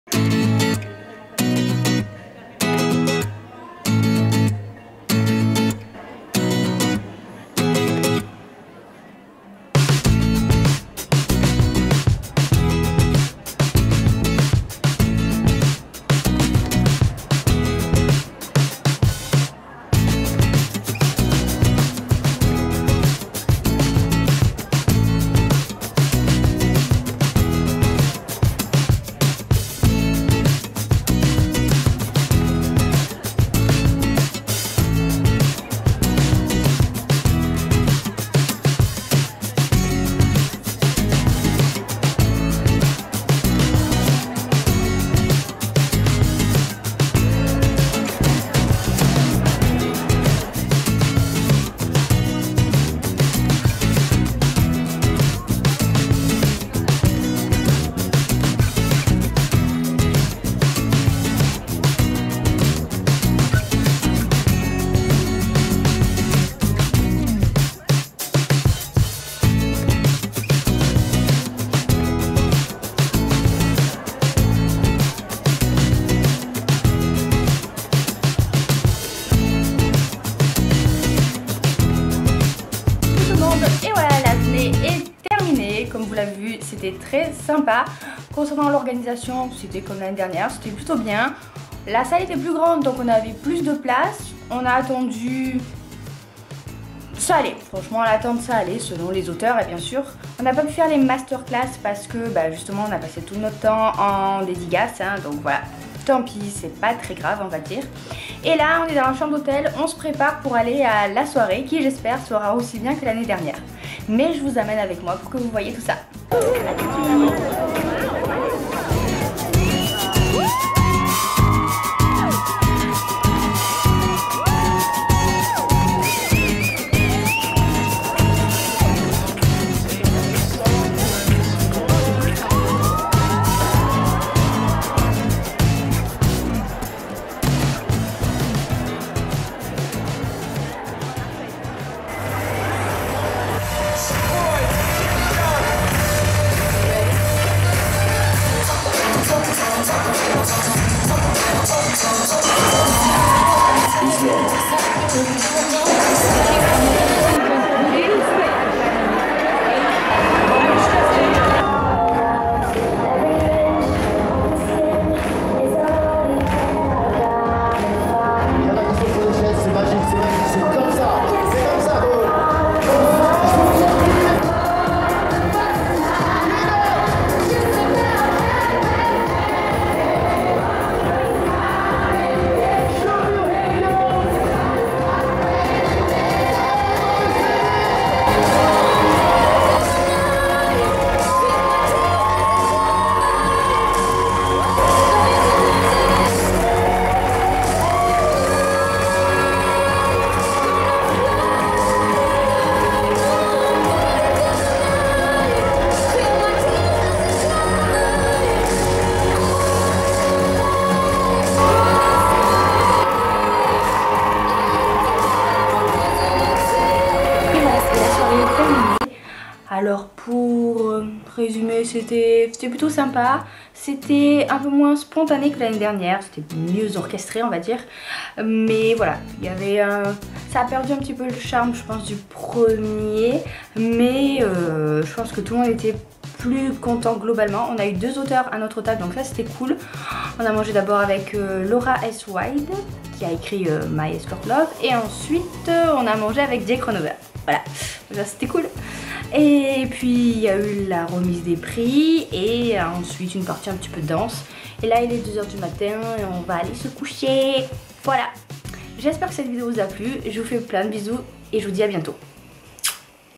Vlog - Festival New Romance 2017
Je vous emmène avec moi au Festival New Romance 2017 à Cannes, pour un petit vlog sans prétention ! ;)

⇩⇩⇩ PLUS D'INFOS SUR LA VIDEO⇩⇩⇩ (Et n'oubliez pas de mettre la HD !)

Le forum sur lequel je suis administratrice, n'hésitez pas à nous y rejoindre